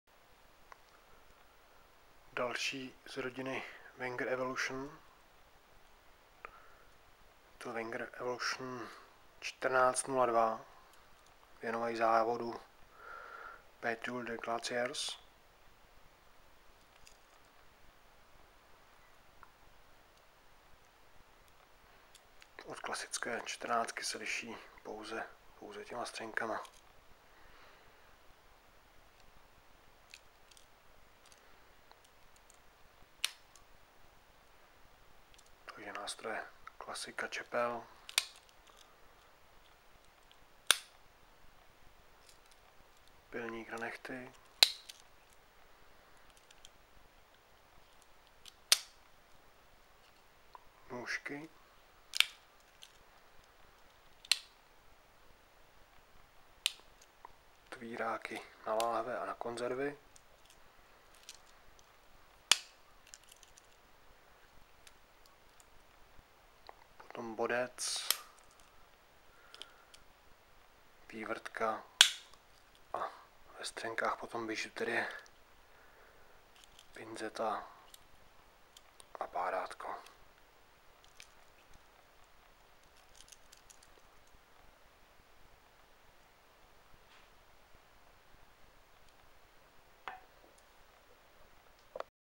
Wenger Evolution 14 PDG
Ukázka nože Wenger Evolution 14 PDG. Koupíte zde